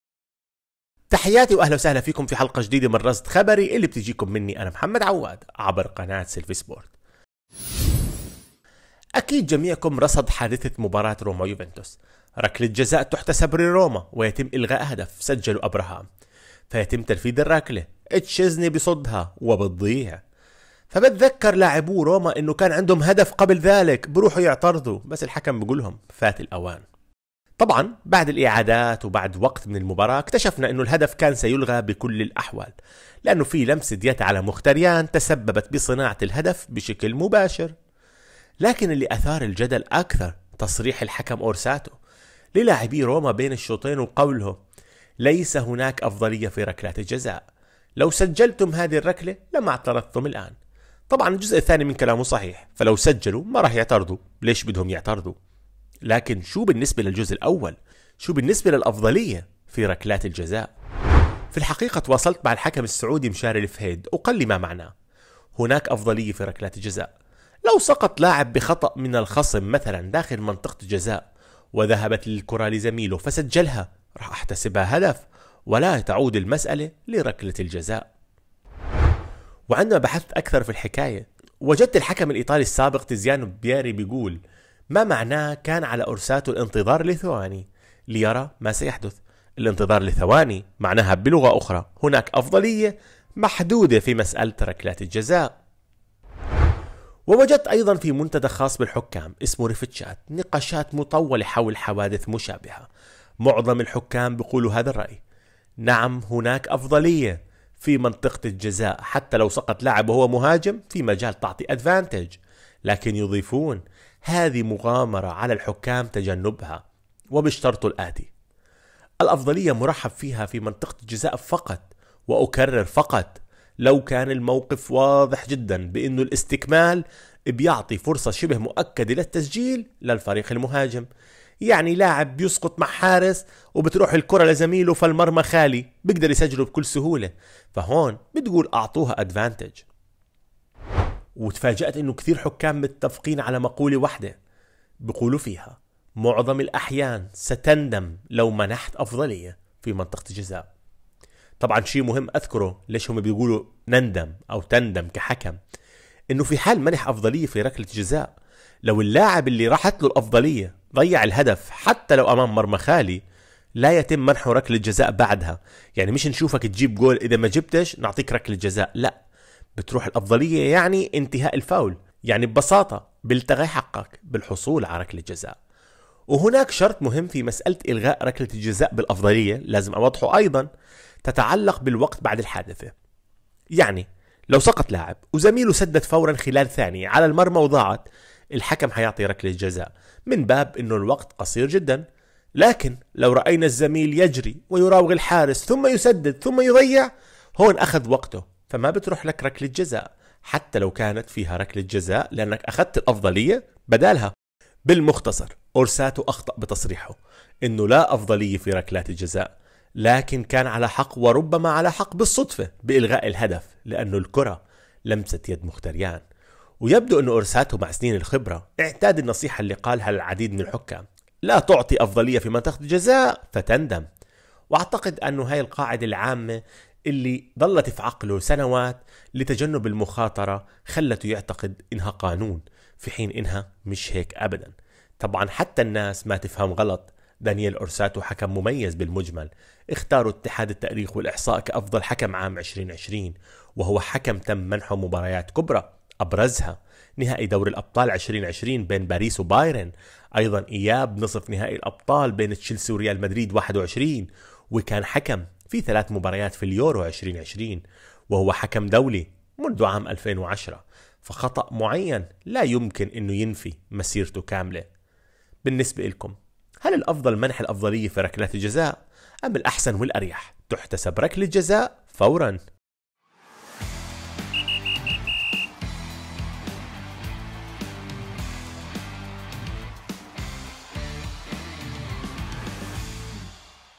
هل هناك أفضلية في ركلات الجزاء؟
هل هناك منح أفضلية في ركلات الجزاء؟

الحقيقة الكاملة بخصوص الأفضلية في ركلات الجزاء

قوانين كرة القدم : الأفضلية في ركلات الجزاء

حساباتي في شبكات التواصل:
تيلغرام :